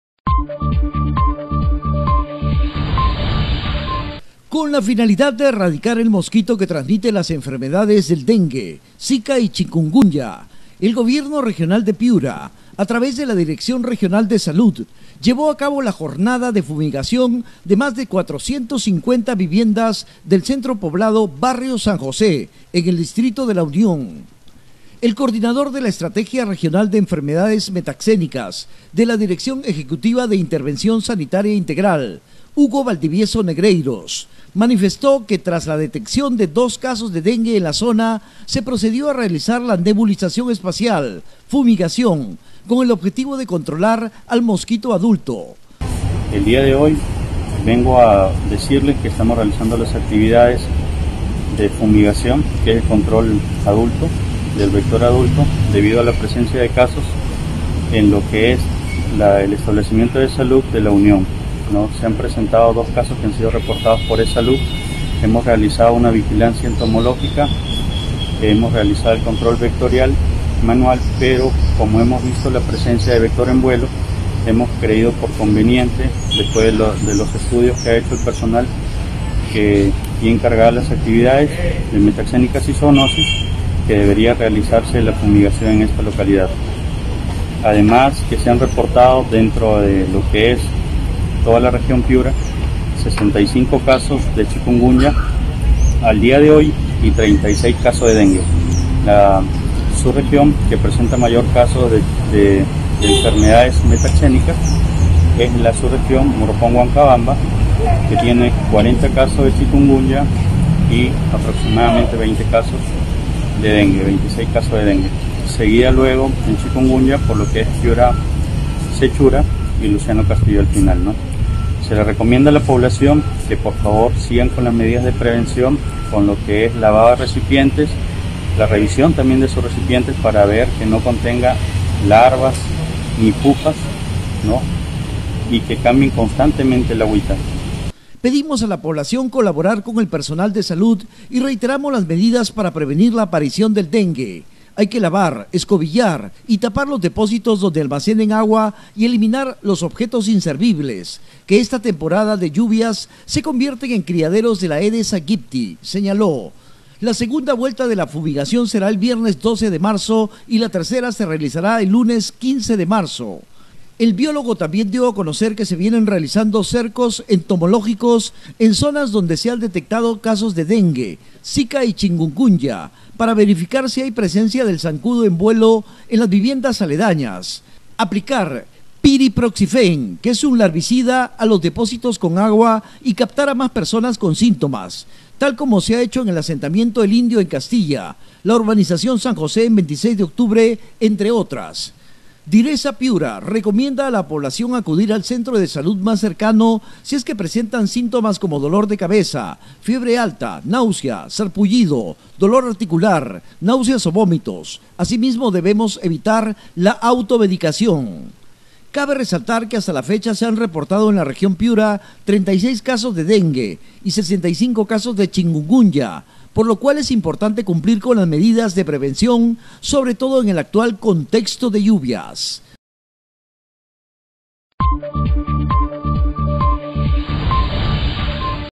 DIRESA FUMIGA MÁS DE 450 VIVIENDAS PARA COMBATIR EL DENGUE EN LA UNIÓN
Con la finalidad de erradicar el mosquito que transmite las enfermedades del dengue, zika y chikungunya, el GobiernoRegionaldePiura, a través de la DirecciónRegionaldeSalud, llevó a cabo la jornada de fumigación de más de 450 viviendas del centro poblado BarrioSanJosé, en el distrito de LaUnión.